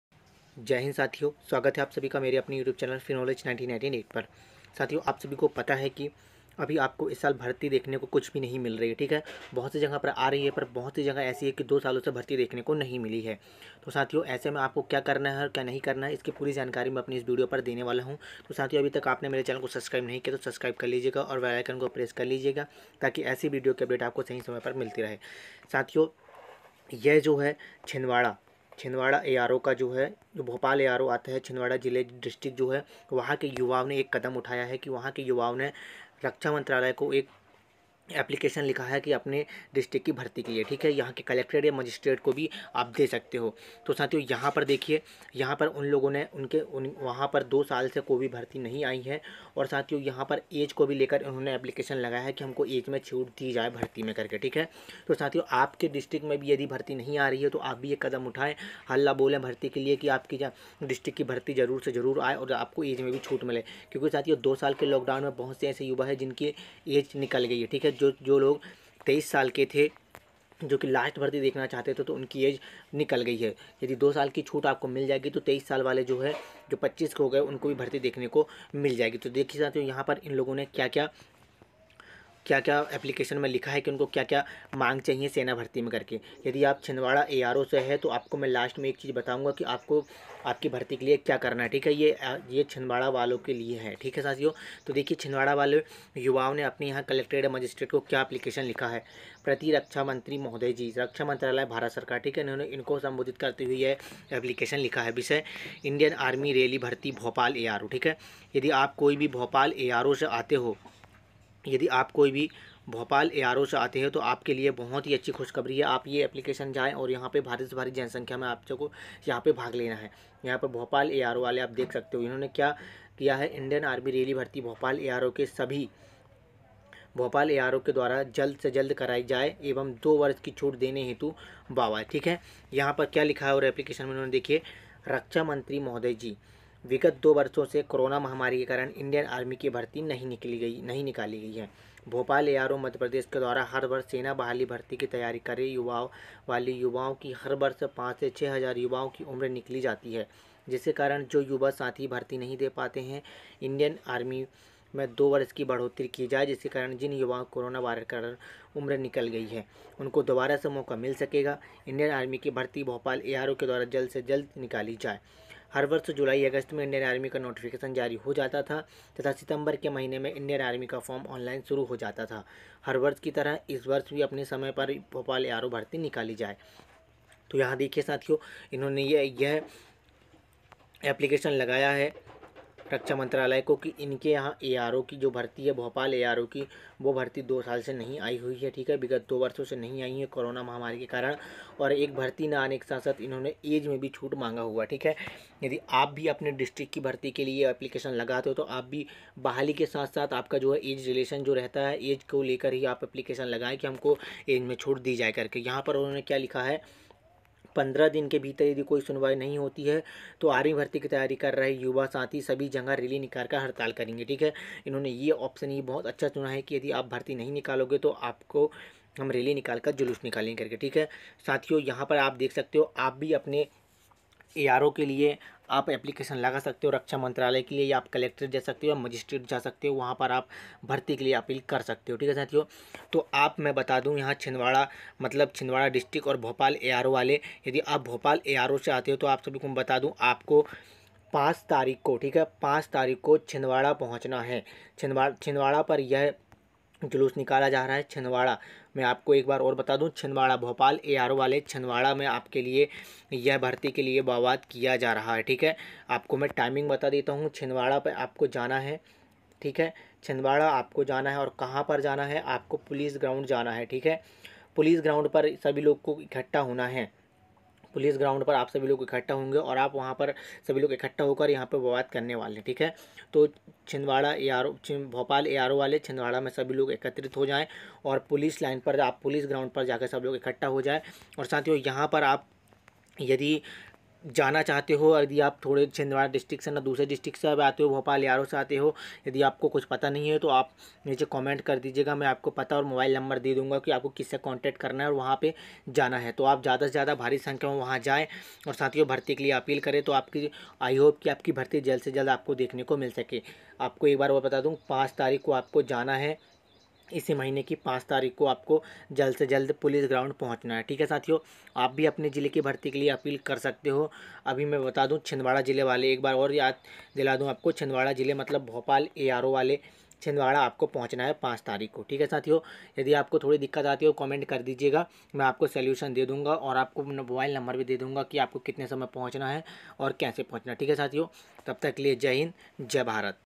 5 अगस्त भोपाल Aro चलो छिंदवाड़ा // सेना भर्ती नही होने के कारण छिंदवाड़ा में रैली // पुलिस ग्राउंड
Hello Everyone welcome to my Free knowledge 1998 YouTube channel

सेना भर्ती ना होने के कारण छिंदवाड़ा में रैली निकाली जा रही है
भोपाल Aro वाले भारी से भारी युवा जाए
छिंदवाड़ा में  पुलिस पुलिस ग्राउंड
छिंदवाड़ा वाले सभी युवा पुलिस ग्राउंड पर पहुंचे और वहां सेना भर्ती ना आने के कारण भारी से भारी संख्या में पहुंचे और समर्थन करें

DONT FORGET LIKE ,SUBSCRIBR, SHARE AND COMMENTS